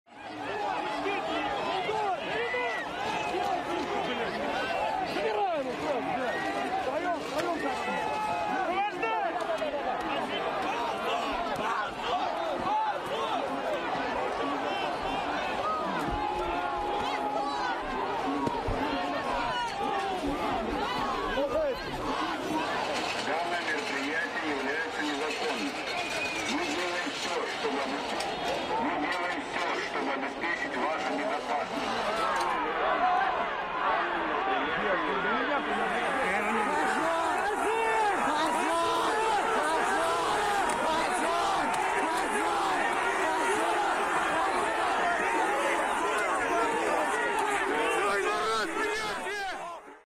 Puschkin-Platz in Moskau: Kundgebung für Nawalny
Der Oppositionspolitiker sitzt in Haft, seine Anhängerschaft fordert die Freilassung.